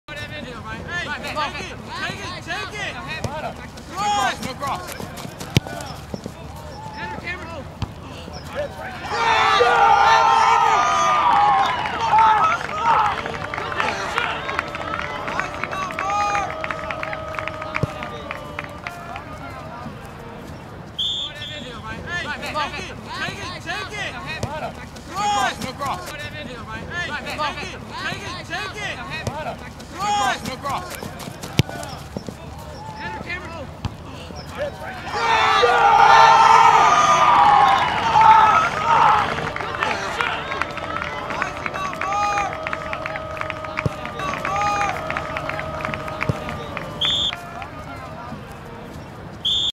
HardingAcademy_Soccer_Gilbert_Avery_Score_3.6.13
Check out this assist (Evan Gilbert) and score (Cameron Avery) by two Harding Academy senior soccer players. This took place in the final second of the first half of action against FCA in a scrimmage on a COLD night (3.6.13) on Cherry Road in East Memphis. Harding went on to win the game by the close score of 2-1.